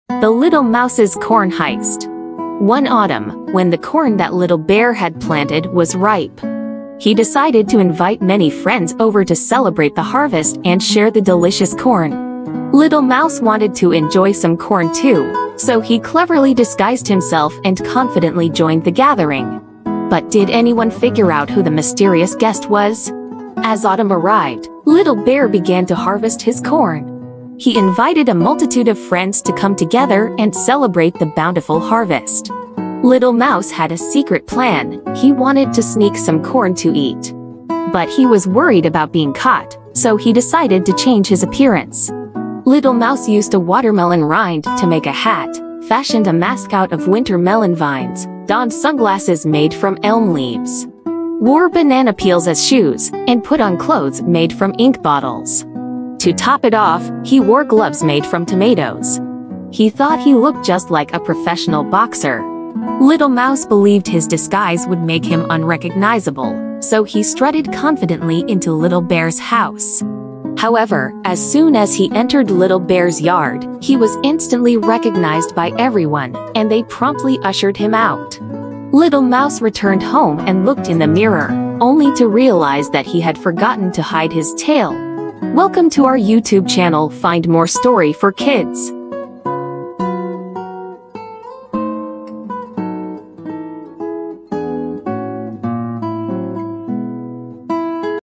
story for kids - The Little Mouse Corn Heist
One autumn, when the corn that Little Bear had planted was ripe, he decided to invite many friends over to celebrate the harvest and share the delicious corn. Little Mouse wanted to enjoy some corn too, so he cleverly disguised himself and confidently joined the gathering. But did anyone figure out who the mysterious guest was?

As autumn arrived, Little Bear began to harvest his corn.

He invited a multitude of friends to come together and celebrate the bountiful harvest.

Little Mouse had a secret plan: he wanted to sneak some corn to eat, but he was worried about being caught, so he decided to change his appearance.

Little Mouse used a watermelon rind to make a hat, fashioned a mask out of winter melon vines, donned sunglasses made from elm leaves, wore banana peels as shoes, and put on clothes made from ink bottles. To top it off, he wore gloves made from tomatoes.

He thought he looked just like a professional boxer.

Little Mouse believed his disguise would make him unrecognizable, so he strutted confidently into Little Bear's house.

However, as soon as he entered Little Bear's yard, he was instantly recognized by everyone, and they promptly ushered him out.

Little Mouse returned home and looked in the mirror, only to realize that he had forgotten to hide his tail.